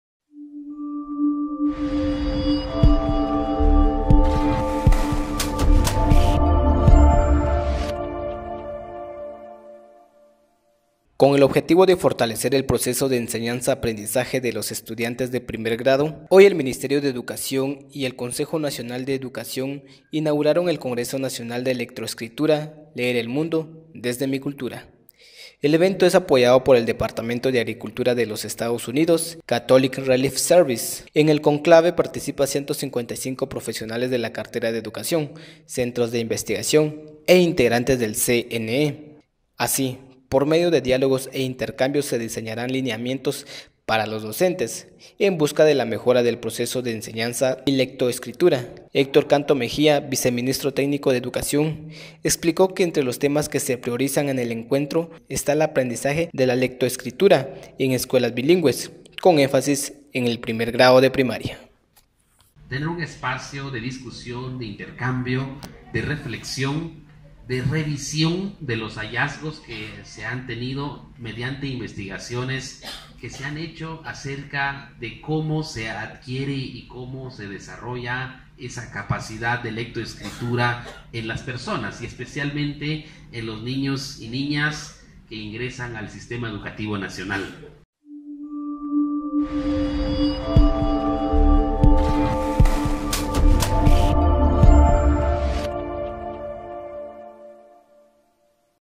Mineduc aboga por programas de lectoescritura para la mejora de la enseñanza
Con el objetivo de fortalecer el proceso de enseñanza-aprendizaje de los estudiantes de primer grado, hoy el Ministerio de Educación (Mineduc) y el Consejo Nacional de Educación (CNE) inauguraron el Congreso Nacional de Lectoescritura “Leer el mundo desde mi cultura”.

El evento es apoyado por el departamento de Agricultura de los Estados Unidos (USDA) y la agencia internacional Catholic Relief Services (CRS).

En el cónclave participan 155 profesionales de la cartera de educación, centros de investigación y e integrantes del CNE

Así, por medio de diálogos e intercambios se diseñaran lineamientos para los docentes, en busca de la mejora del proceso de enseñanza y lectoescritura.

Héctor Canto Mejía, viceministro técnico de educación, explicó que entre los temas que se priorizan en el encuentro está el aprendizaje de la lectoescritura en escuelas bilingües, con énfasis en el primer grado de primaria.